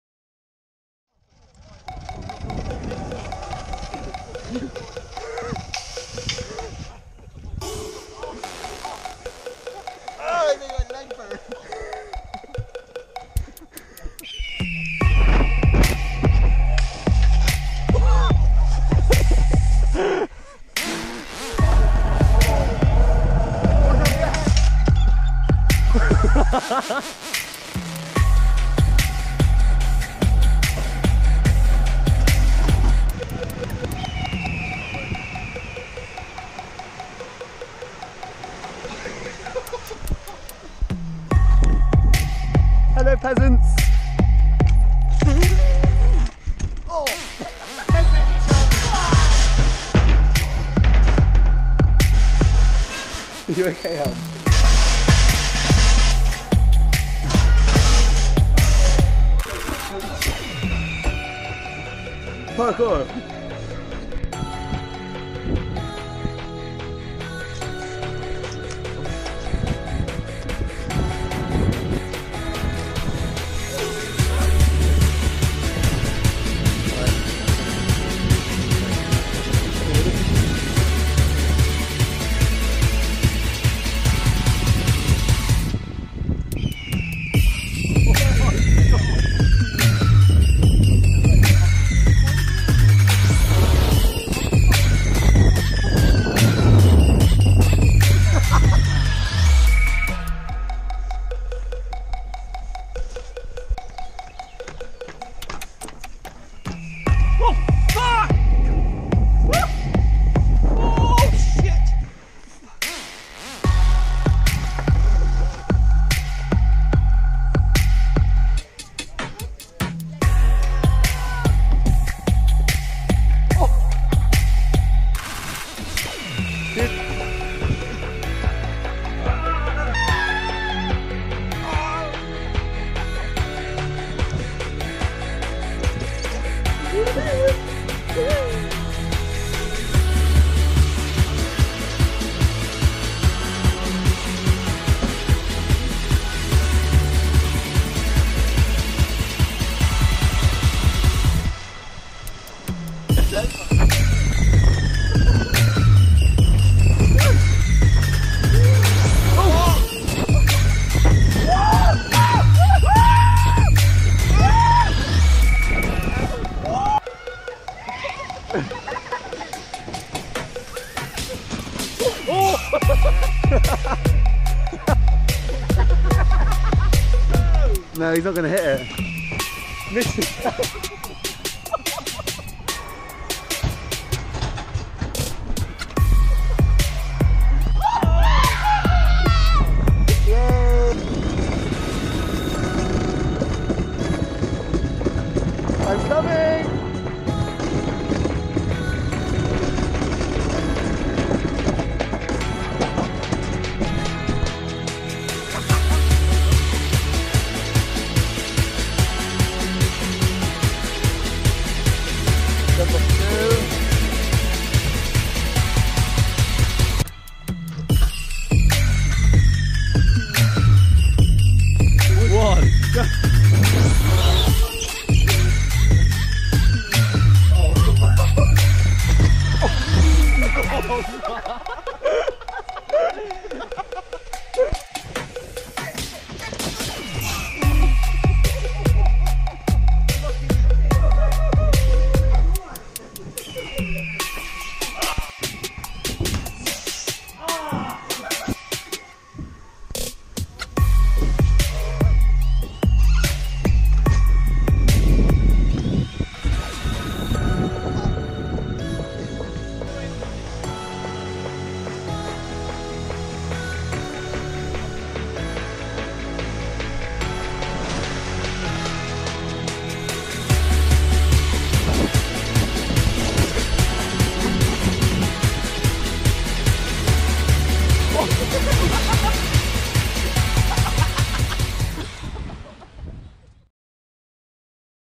Woodlands May Day Bank Holiday Weekend
A sweet cheeky edit from a great weekend being big kids at Woodlands in Devon!